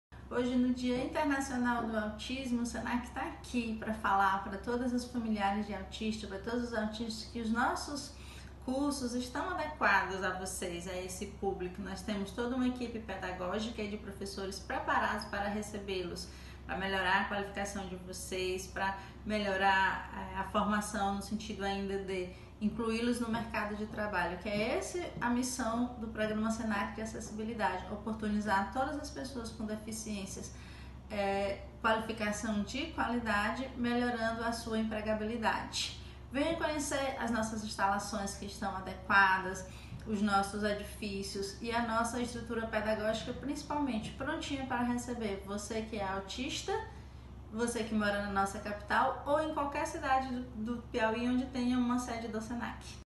O Senac Piauí, Autistas e Mercado de Trabalho
No Dia Mundial de Conscientização do Autismo, a Coordenadora de Promoção Social do Senac Piauí, Regina Ramos, relembra que estamos não só aptos, mas de braços abertos para receber autistas nas nossas turmas, com o objetivo de qualificá-los e prepará-los para o mercado de trabalho.
Os profissionais do Senac, em parceria com o Ministério Público do Trabalho e com a Escola Viva, tiveram seus supervisores e instrutores qualificados para realizar a profissionalização de autistas. Estamos crescendo e queremos que você cresça junto!